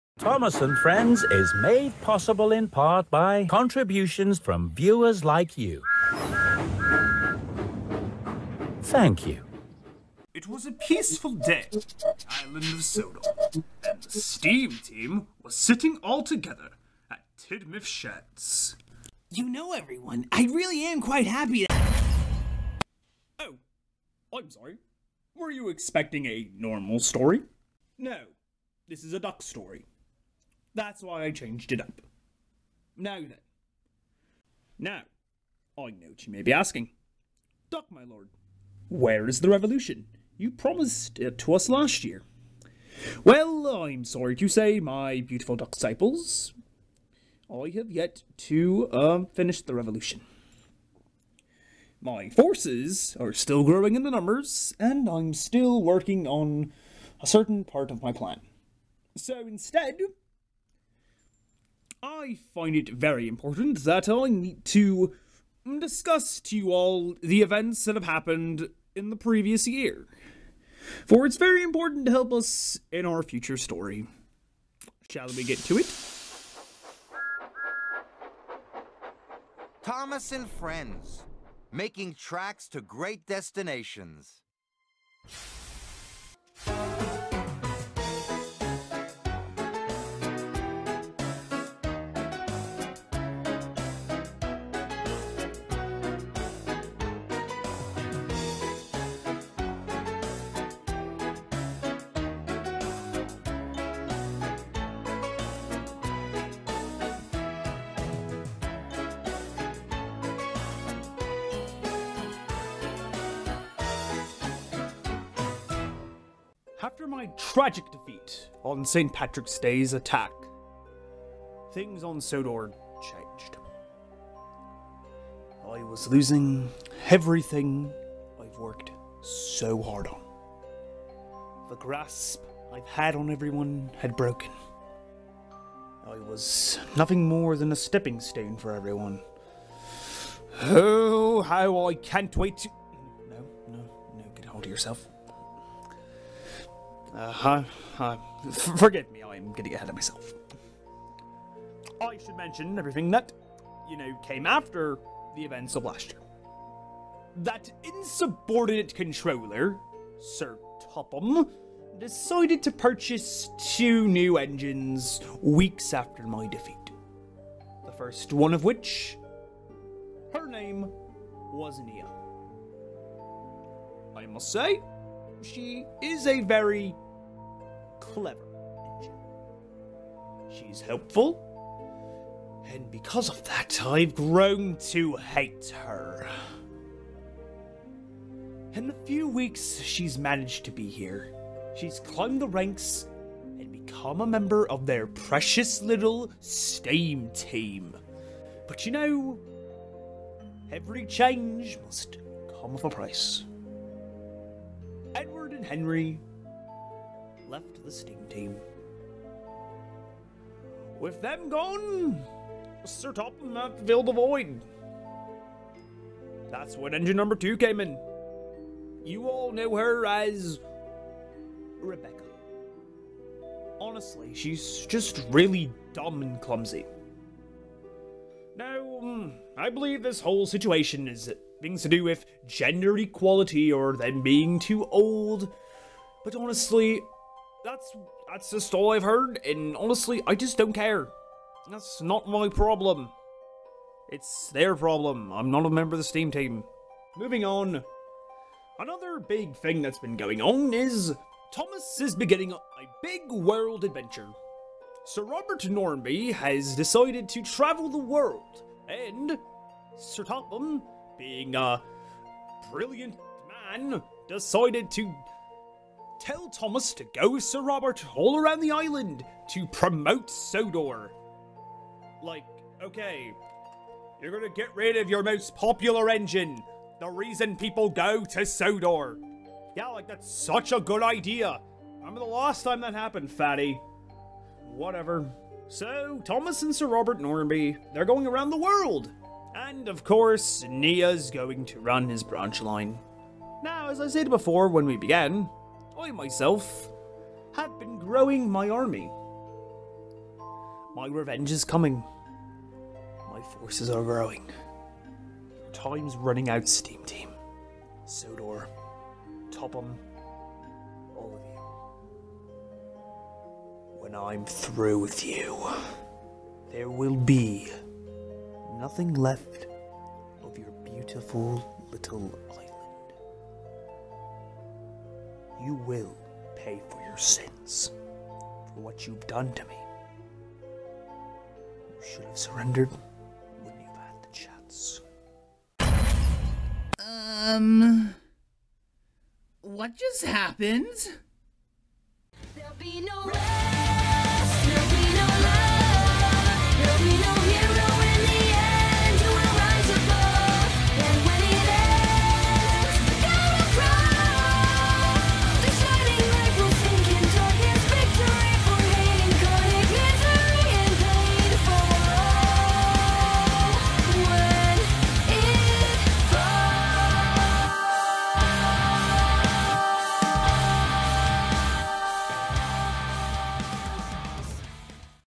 Thomas Trackmaster Adventures Classic Short 8.5
one year after the events of ducks failed 2018 plans, we learn for the man himself about what ha happened on sodor and maybe learn of his plans.

this video mainly just exist to establish nia and rebecca and to fill the wait  for duck revolution.

next year, i promise